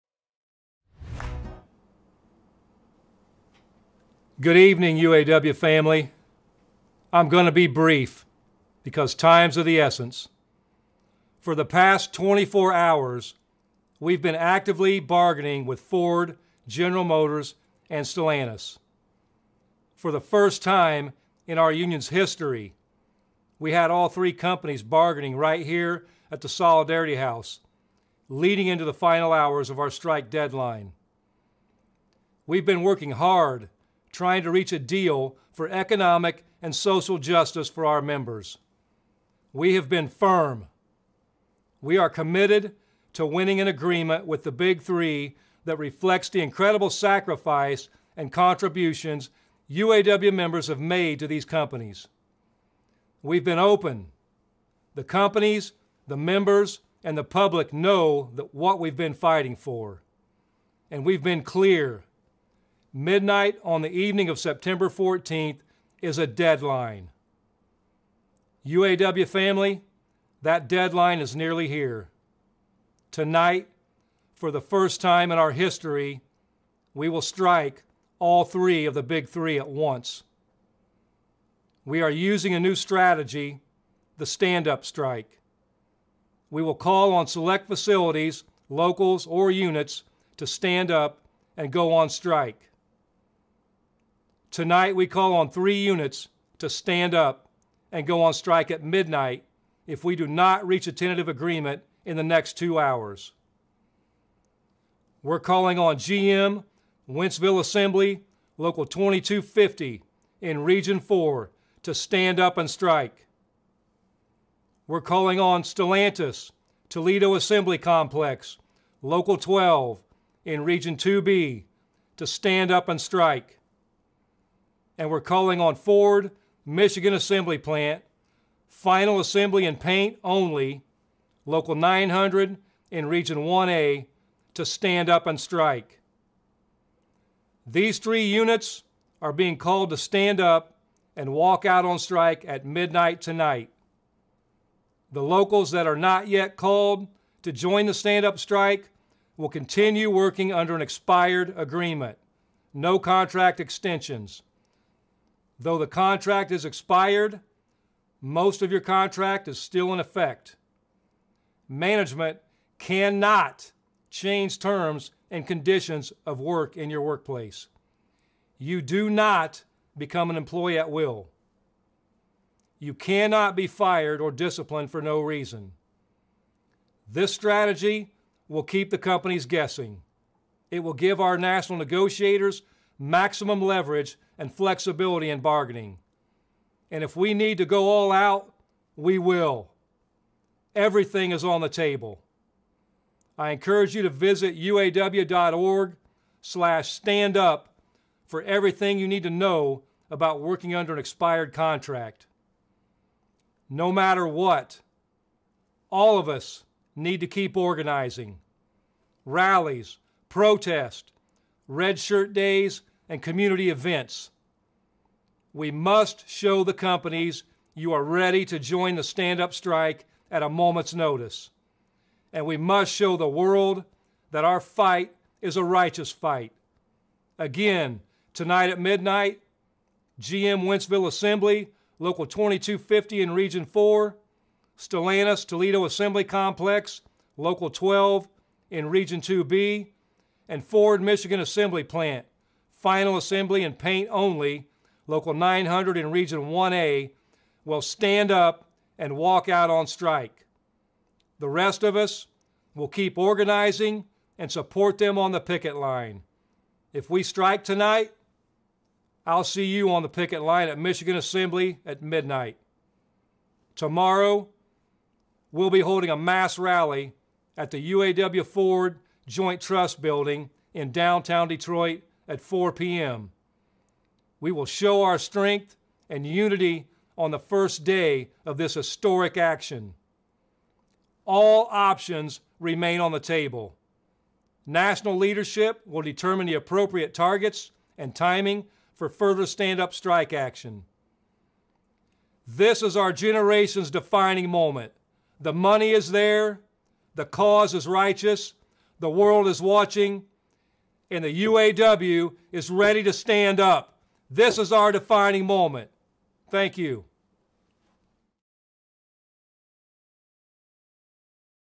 Invest Into US Families - UAW President Calls a Strike of All 3 Automakers.
UAW (ALA) Alliance of Labor Action